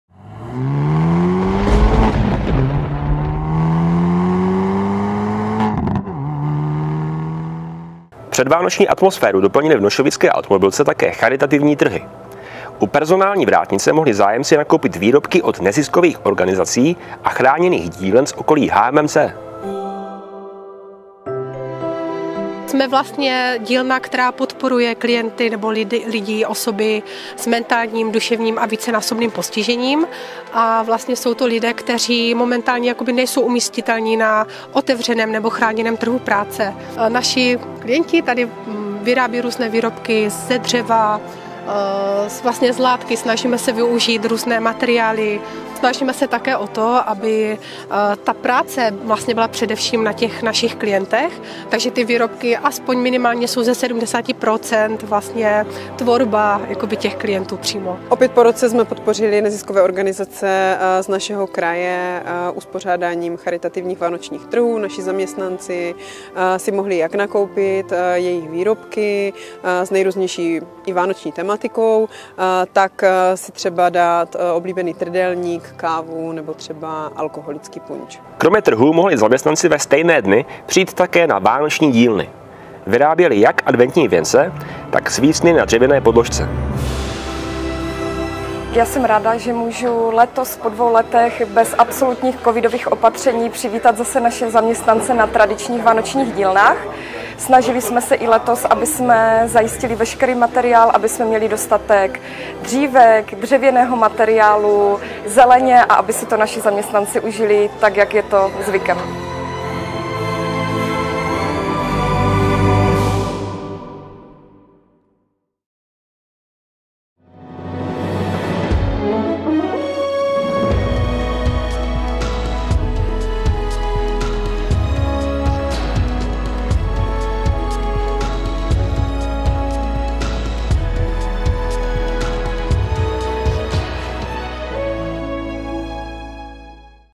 HMMC TV 86/2022 | Vánoční trhy a dílny
Předvánoční atmosféru doplnily v nošovické automobilce Hyundai také charitativní trhy a tradiční vánoční dílny pro zaměstnance.